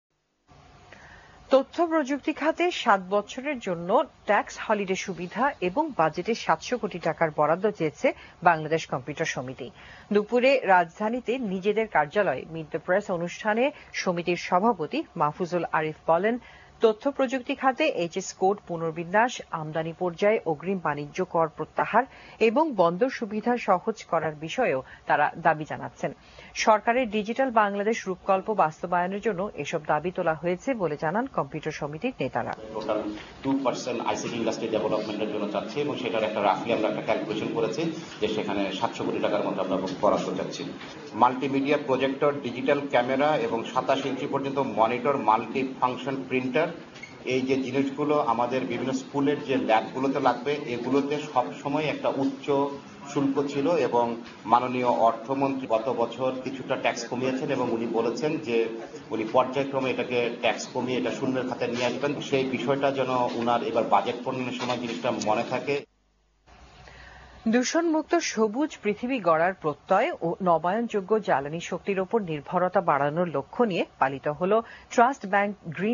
Meet the Press on Discussion with Newly Elected BCS EC & Upcoming Budget 2014-2015 in ATN News
BCS organized a Meet the Press on Discussion with Newly Elected BCS Executive Committee & Upcoming National Budget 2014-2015 at BCS Secretariat on 15 April 2014. BCS President Mr. A.H.M Mahfuzul Arif was spoke.